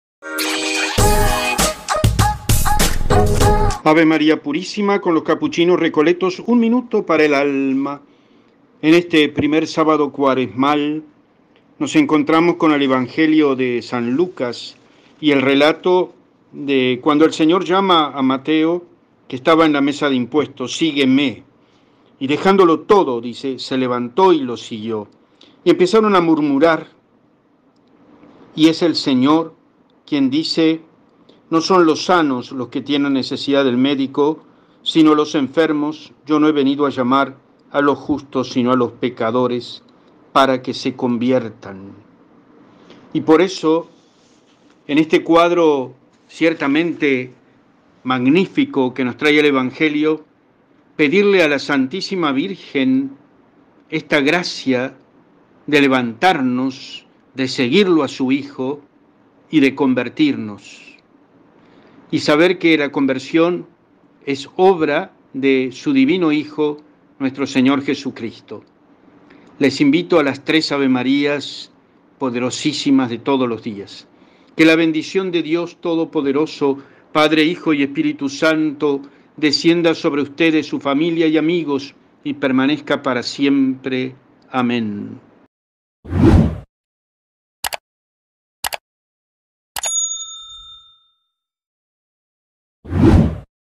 Evangelio de San Lucas. Capuchinos recoletos. (17/02/24) - PASOS DEL CREYENTE
Evangelio de San Lucas, es la oración de los frailes Capuchino recoletos del día de hoy, sábado 17 de febrero